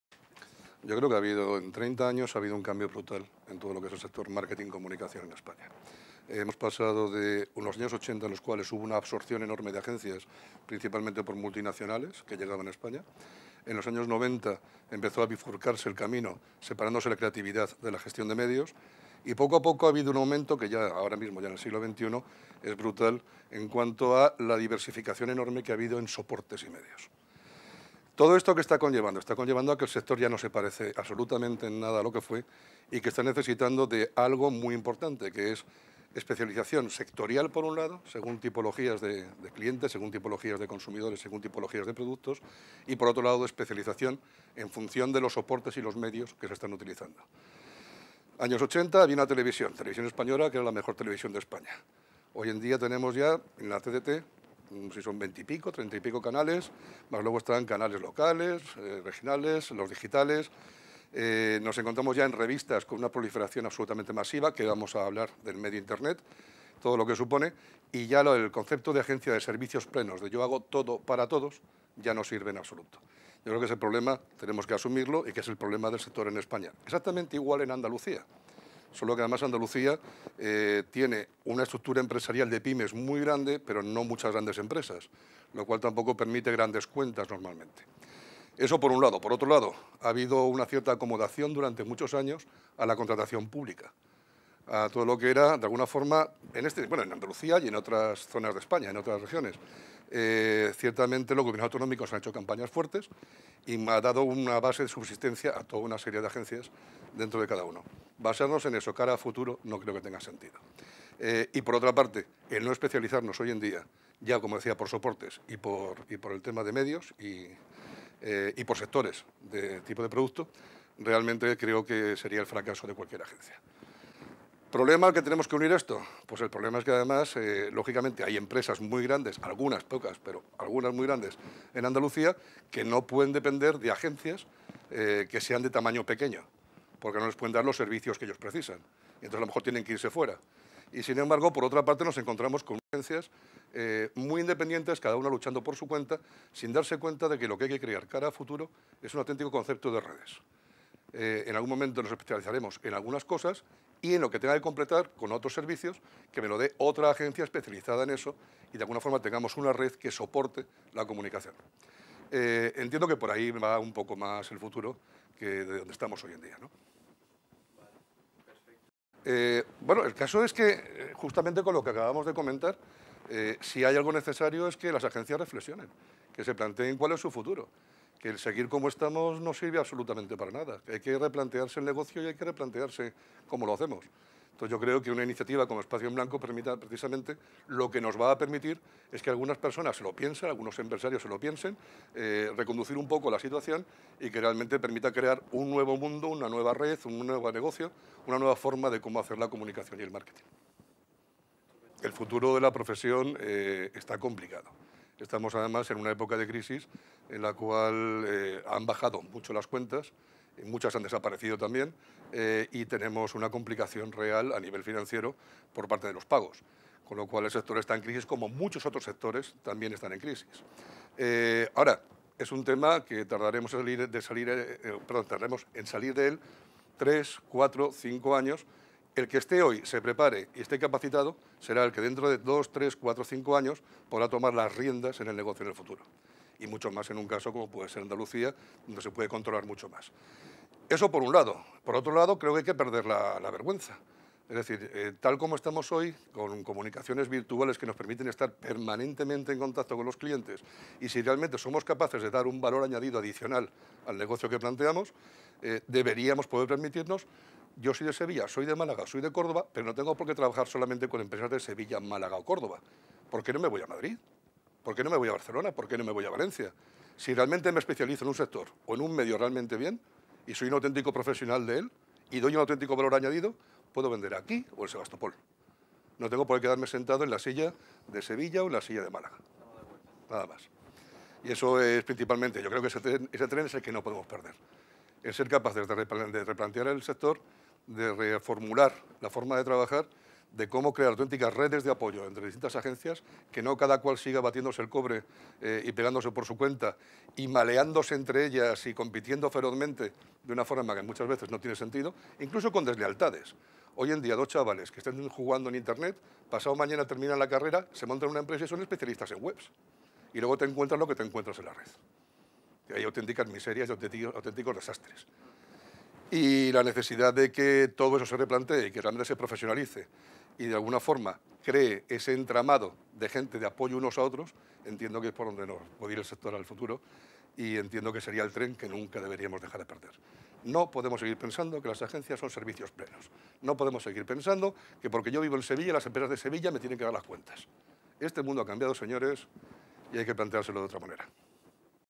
Vision del Sector Publicitario en España | Javier Pezonaga
Javier Pezonaga, Presidente Mel Interbrand, nos da su visión sobre el Sector Publicitario en España para las Jornadas "Espacio en Blanco".